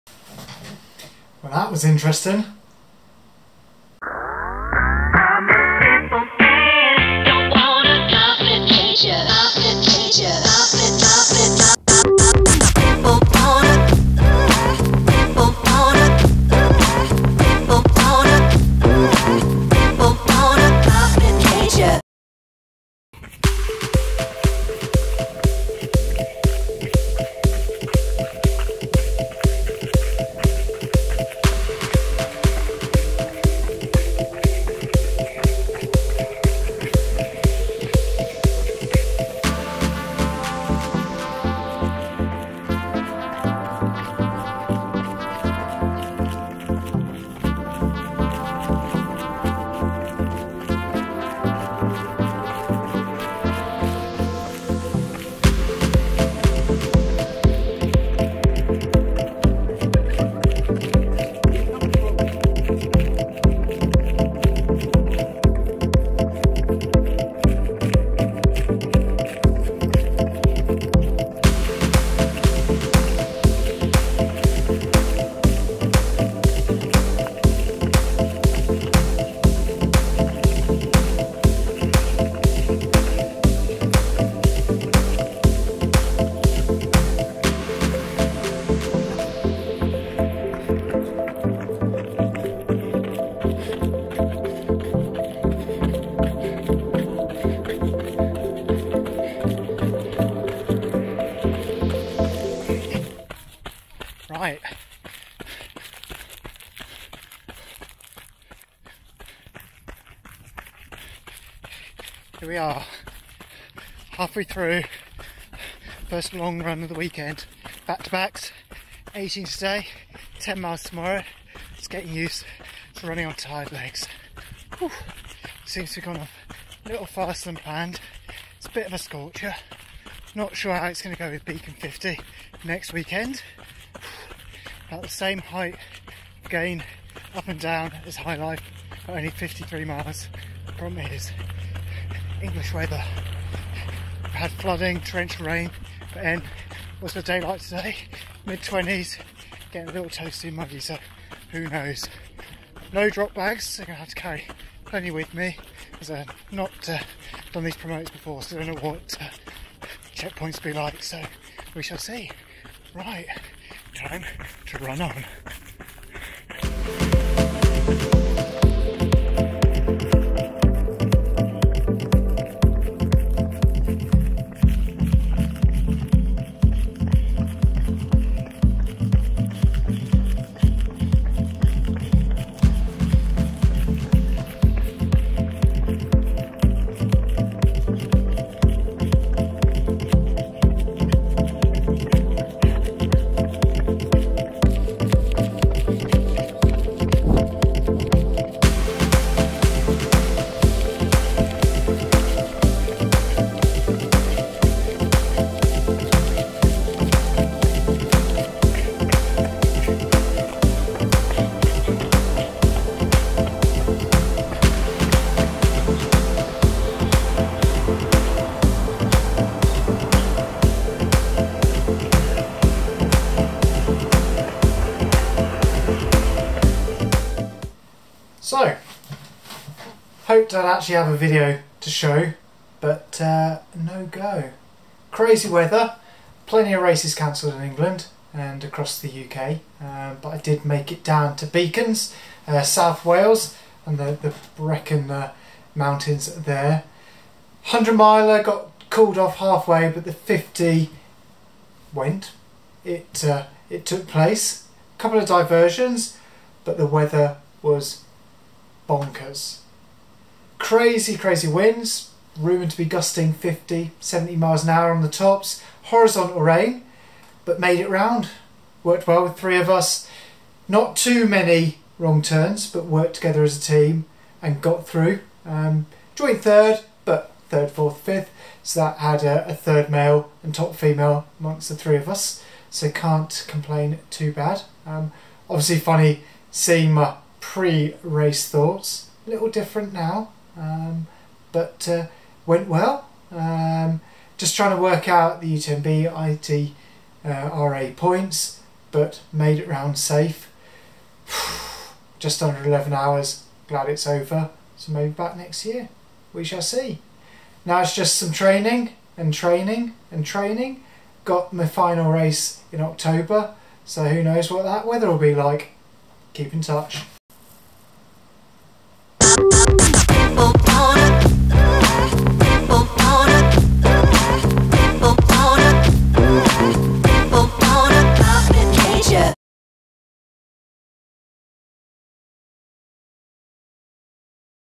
VLOG 1.2 - Beacons 50 Ultra - Thoughts before & after
At the weekend I raced in the Beacons 50, an ultramarathon over 50 miles in length with over 10,000ft in ascent in South Wales, UK. I recorded some thoughts on a training run before and captured some thoughts after.
Torrential rain, 50+mph winds and tough underfoot. It was going to be a tough and long day but I came 4th (joint 3rd). Weather was horrible and safety a priority so no film or photos taken during the race sadly. Training continues...